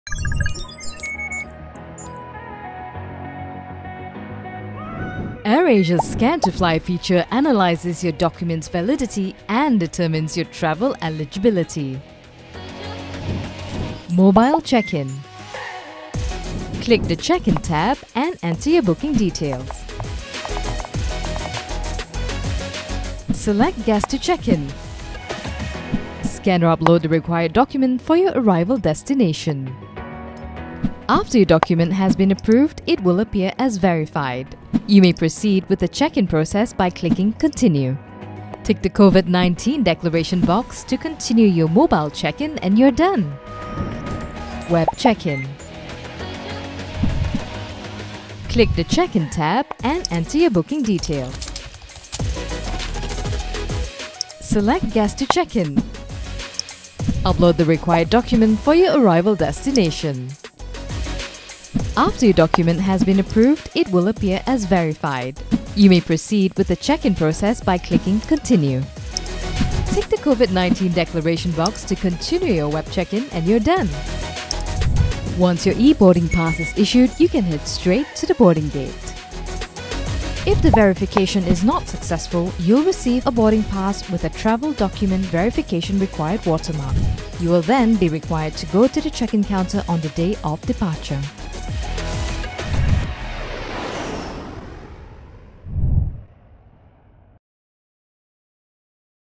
AirAsia | How to use Scan2Fly to verify Covid-19 related travel documents instantly
Verify country-specific Covid-19 related travel documents instantly in a few simple steps via mobile or web, before heading to the airport with Scan2Fly.